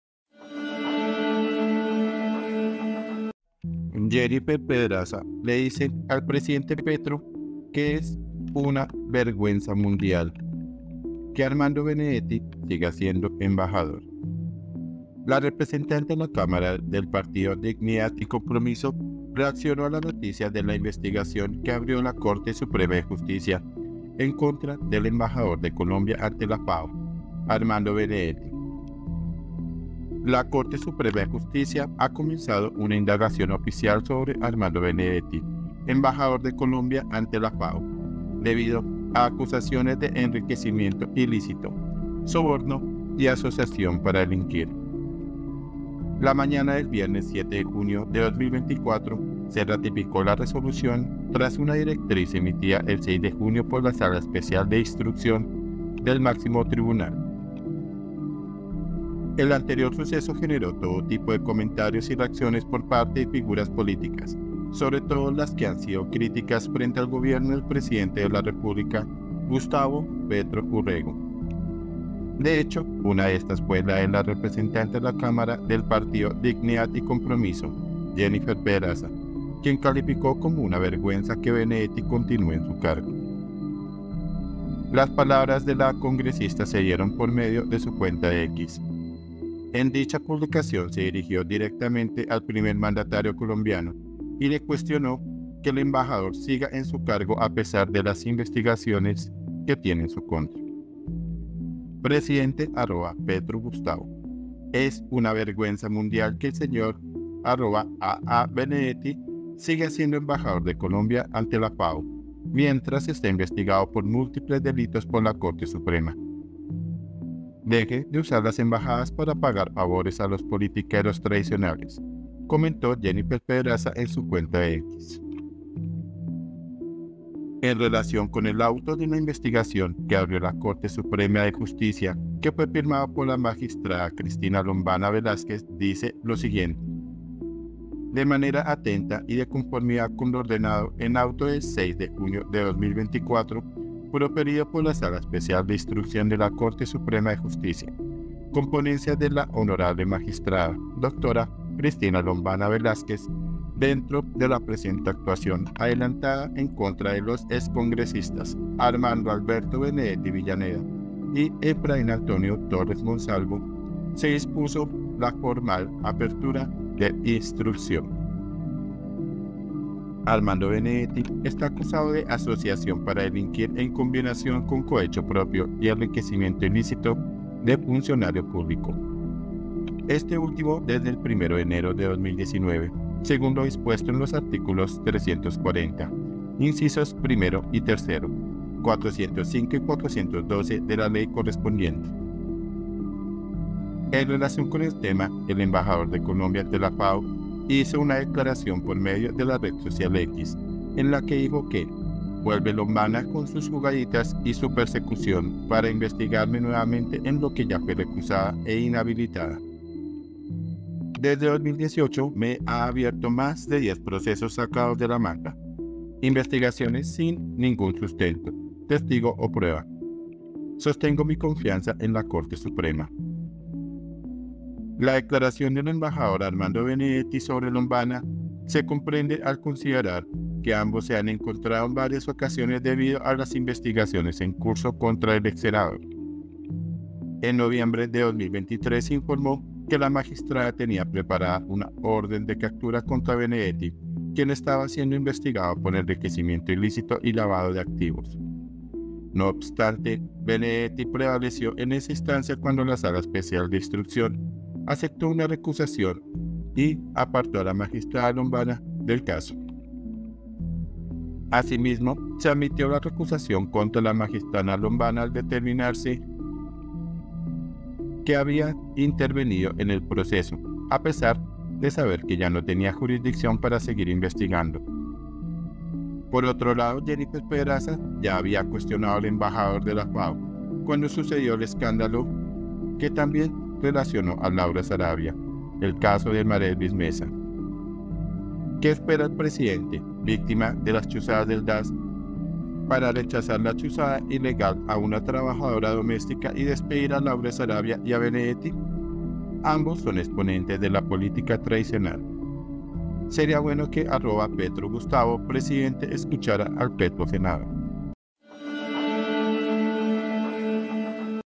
"VERGÜENZA MUNDIAL" 📢 JENNIFER PEDRAZA A PETRO POR ARMANDO BENEDETTI
Jennifer Pedraza le dice al presidente Petro que es " una vergüenza mundial” que Armando Benedetti siga siendo embajador
La representante a la Cámara del partido Dignidad y Compromiso reaccionó a la noticia de la investigación que abrió la Corte Suprema de Justicia en contra del embajador de Colombia ante la FAO, Armando Benedetti
INFOBAE